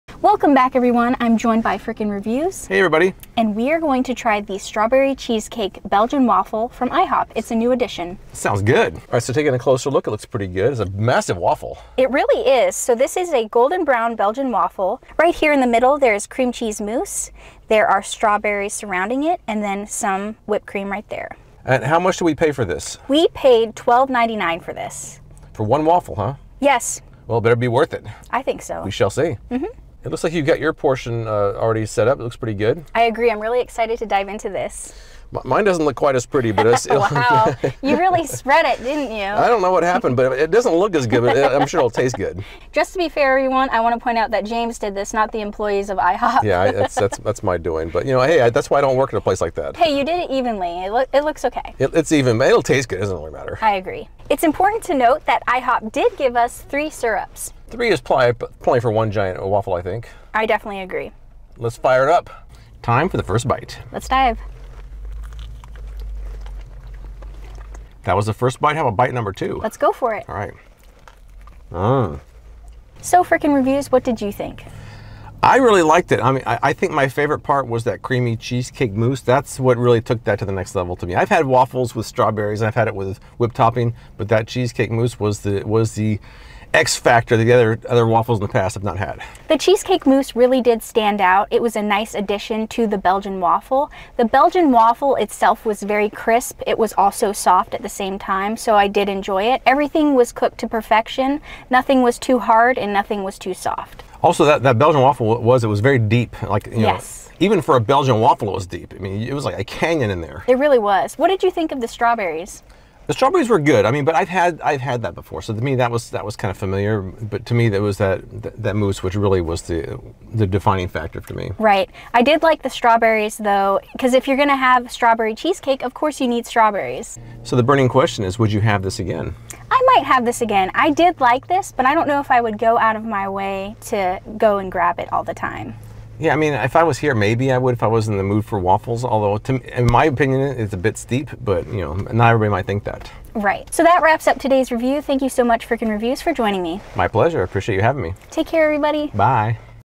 Taste Test: IHOP's NEW Strawberry Cheesecake Waffle Edition!
Join us as we dive into IHOP's latest creation: the Strawberry Cheesecake Belgian Waffle! In this review, we'll provide an in-depth analysis of IHOP's Strawberry Cheesecake Belgian Waffle, offering our honest opinions on its taste, texture, and presentation.